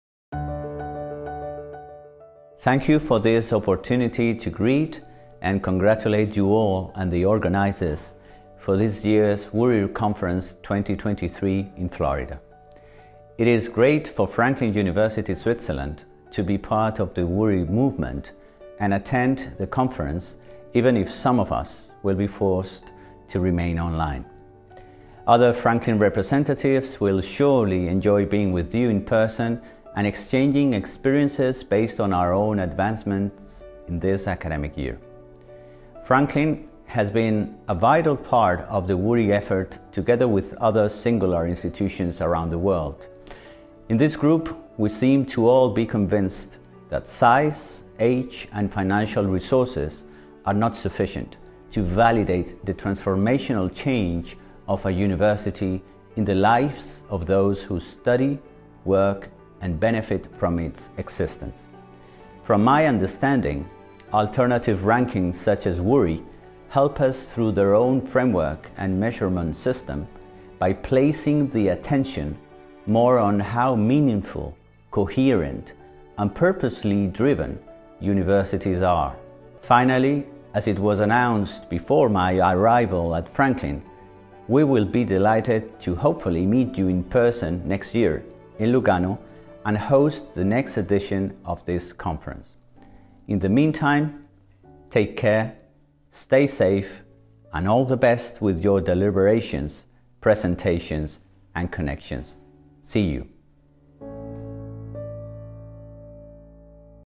Franklin and WURI Raking: a message from President Martín-Barbero
In the fourth edition of the World's Universities with Real Impact (WURI) ranking system, FranklinUniversitySwitzerland placed 26th in the top 100 most InnovativeUniversities. The university excelled in several specialty rankings, including #3 in the top 50 for EthicalValue, #4 in the top 50 for CrisisManagement, and #15 in the top 50 for IndustrialApplication.
What sets the WURI ranking apart is its focus on universities' efforts in engaging with and improving the future, environment, and society through innovative contributions and creative approaches. The ranking system acknowledges the challenges of creating new roles and responsibilities in leading and catalyzing change and impact, towards economic prosperity and social harmony, especially during the latest global challenges of the emergence of COVID-19 and the spread of the industrial revolution. Lastly, it promotes cross-learning and innovation among all universities.

Watch the video to hear more from President Samuel Martín-Barbero about Franklin University Switzerland and WURI ranking system 👆